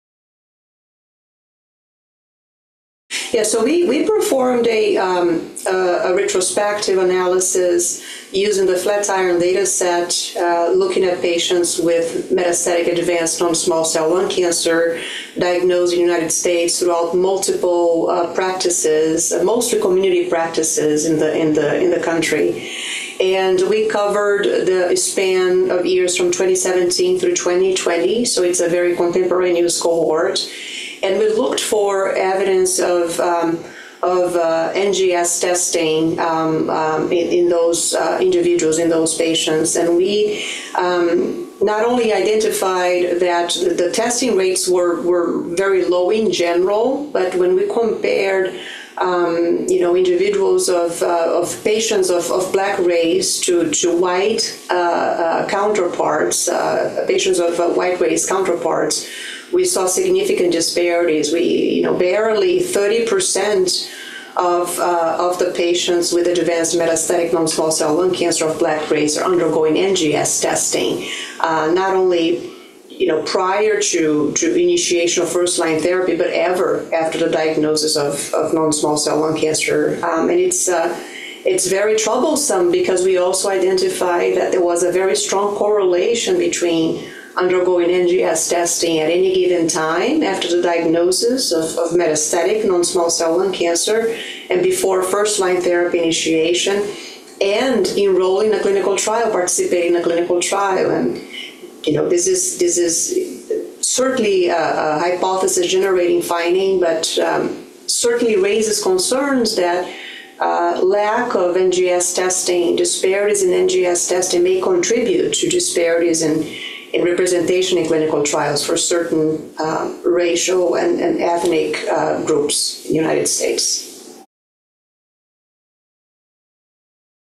Racial disparities in lung cancer biomarker testing and clinical trial recruitment
Debora Bruno, MD, Seidman Cancer Center, Cleveland, OH, comments on findings from an analysis of a database, which revealed low rates of next-generation sequencing (NGS) in patients with lung cancer. Testing rates were especially low in patients of color, where black patients with non-small cell lung cancer (NSCLC) were subsequently underrepresented in clinical trials. This interview took place at 2024 Targeted Therapies of Lung Cancer Meeting (TTLC 2024) in Santa Monica, CA.